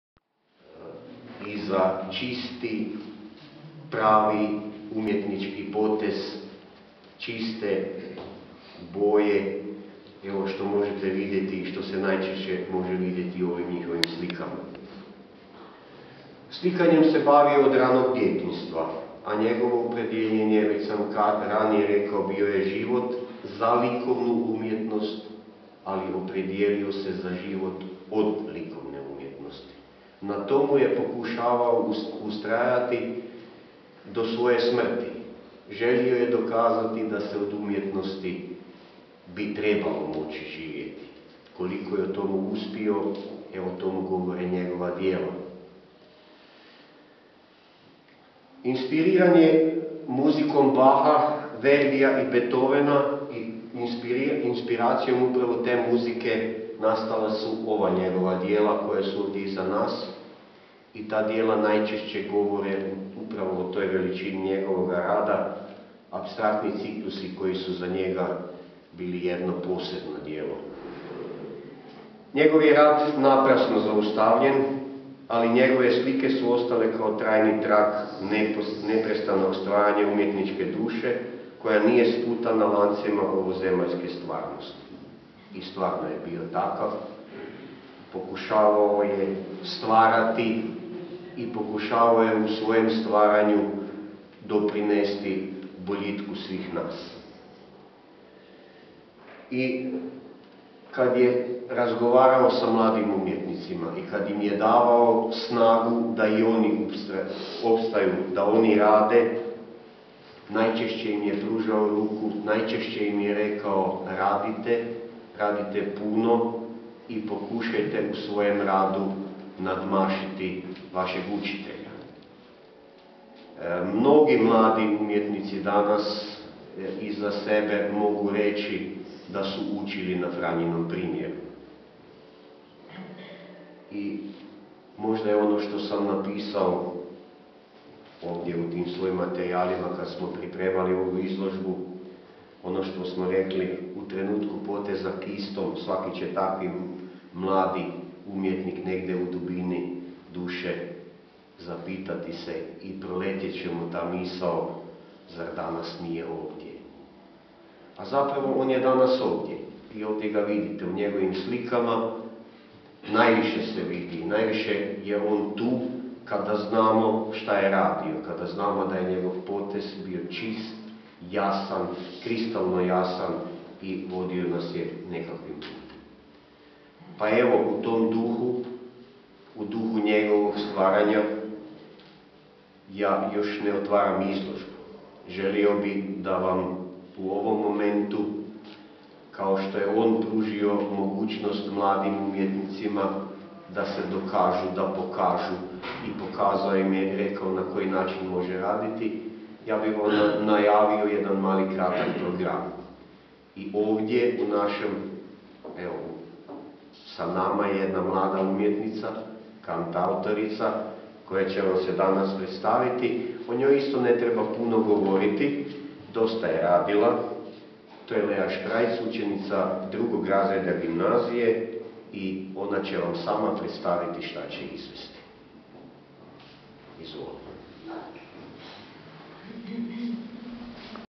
Franjo Horvat - izložba - exhibition
Franjo Horvat rođen 22. svibnja 1959. godine u Varaždinu, umro u Slakovcu 03. prosinca 2011. godine. Životno je vezan za Šenkovec , gdje je proveo djetinjstvo, Slakovec , gdje je stvarao i Čakovec, u kojem je proveo bezbrojne dane promovirajući kulturu življenja. Završio je Školu primijenjene umjetnosti u Zagrebu -- smjer slikarstvo. Slikanjem se bavi od ranog djetinjstva, a njegovo opredjeljenje je bilo život za likovnu umjetnost i život od nje. Za sebe je govorio, da živi u svojim motivima, motivima Hrvatske, koji su predivni i da voli raditi u maniri starih majstora slikarstva, flamanske i haške škole. Inspiriran muzikom Bacha, Verdia, Beethovena , itd, stvorio je apstraktne cikluse slika, koji odišu traganjem za istinom o beskraju, vječnosti i duhovnosti. Njegov je rad naprasno zaustavljen, ali njegove slike ostale su kao trajni trag neprestanog  stvaranja umjetničke duše, koja nije sputana lancima ovozemaljske stvarnosti. I ne budi samo slika uspomenu na autora, već je budi topla riječ, koja je ostala nakon bezbrojnih kavica čakovečkih kafića i slastičarni . I svakom će  mladom slikaru ,  kojemu je Franjo pružio ruku, podijelio pokoji savjet , u trenutku poteza kistom , negdje u dubini duše , proletjeti misao : « Zar danas nije ovdje? «

                                                                                              Tihomir Benčić

« Iako njegov rad upućuje na simboličku transkripciju suvremenih proživljavanja, on ujedno progovara o suptilnom, emotivnom i profinjenom slikarskom reagiranju koje je znalo naći ravnotežu između nekad krajnosti i uskladiti ih u psihološki snažnu poruku. Splet njegovih promišljanja odvodi i promatrača i  nove sfere duhovnog odnosa prema prikazanom. Promatraču je predočen subjektivan svijet umjetnika koji ne samo da koristi oblike i boje, već u svoje radove ugrađuje atmosferu, duhovno stanje vremena u kojem živimo i na kraju istinu često duboko skrivenu. «
                                                                                         Juraj Baldani

« Koloritom i kompozicijom zaustavlja dah trenutka upravo viđenog. Iz toga crpi snagu za stvaranjem novih oblika viđenja poimanja života i smrti, što autor nastoji dočarati svojim djelima nadrealizma»
                                                                                     Zlatko Baranašić